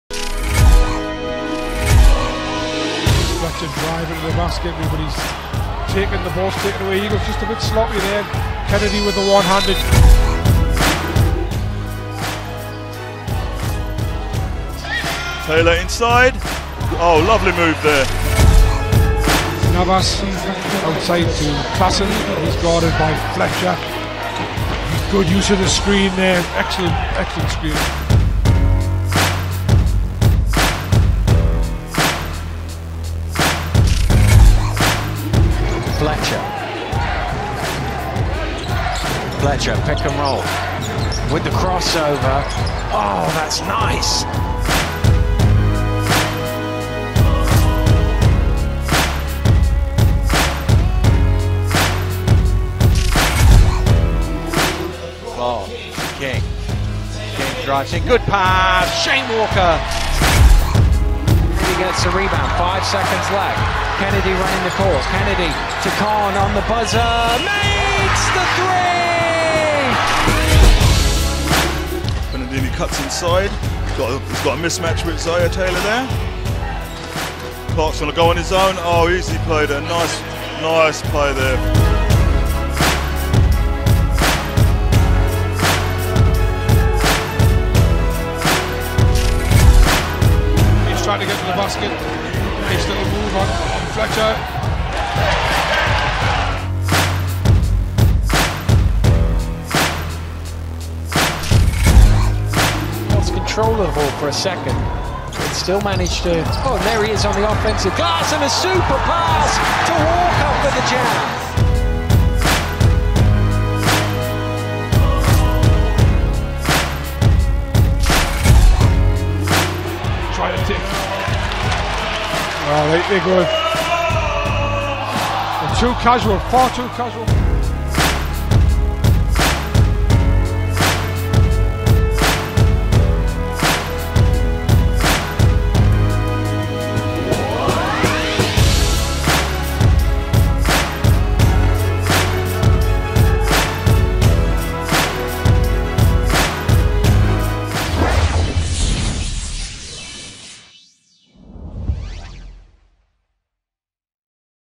Marek Klassen Gets Huge Chasedown Block! BBL Top 10 Plays Week 33
Check out the top 10 plays from Week 33 of the 2016-17 BBL season, led by Worcester Wolves' Marek Klassen with a massive chase down block!

Also featuring: Jermel Kennedy,  Zaire Taylor, Rahmon Fletcher, Shane Walker, Ashton Khan, Brandon Clark and Trevor Setty.